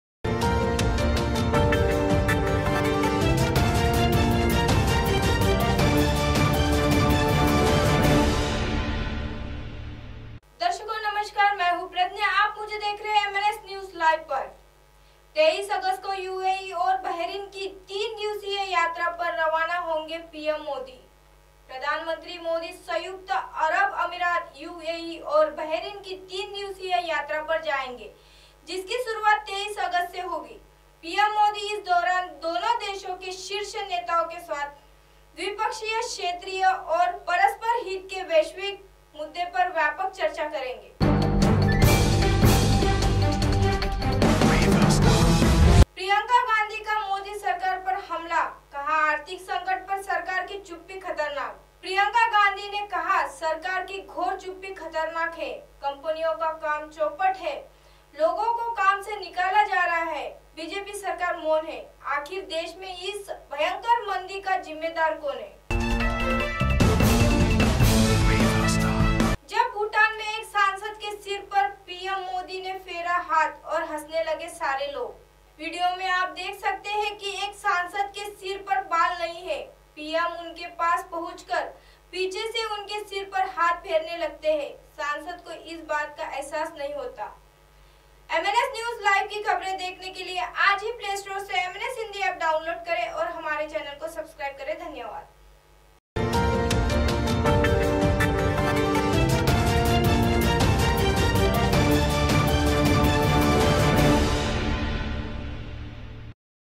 MNS #२३ अगस्त को UAE और बहरीन की तीन दिवसीय यात्रा पर रवाणा होंगे PM मोदी।
MNSNEWSLIVE #२३ अगस्त को UAE और बहरीन की तीन दिवसीय यात्रा पर रवाणा होंगे PM मोदी